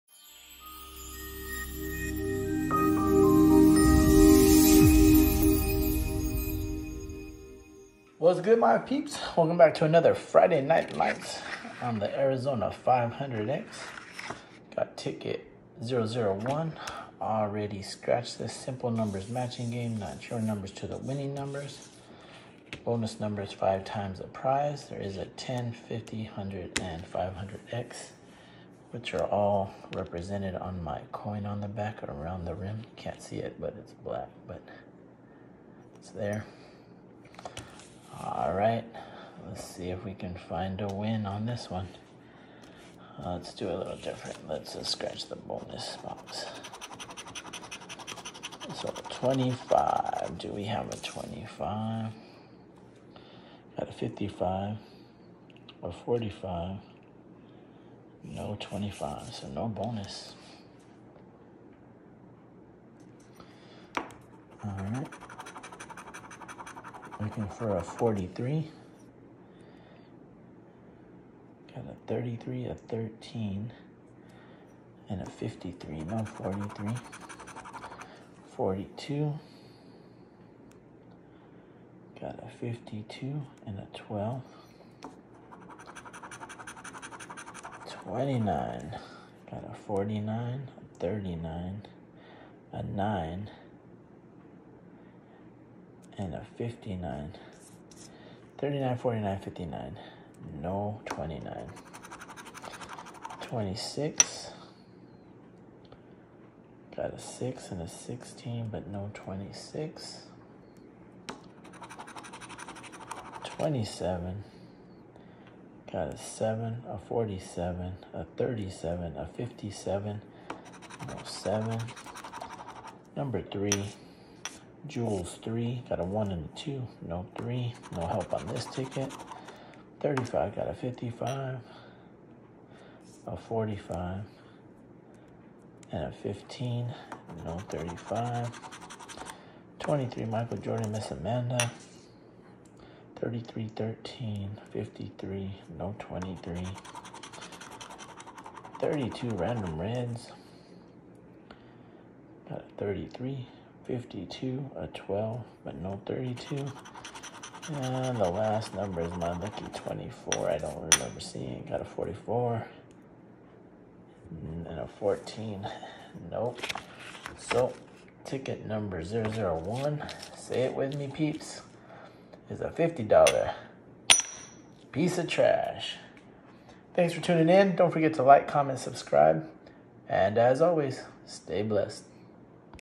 Friday Nite Lites... $50 500X... Ep 16
Episode 16 my peeps... friday nite lites on the $50 Az 500X... thanks for always coming in & showing all the love & support... please don't forget to like. comment... subscribe... & stay blessed.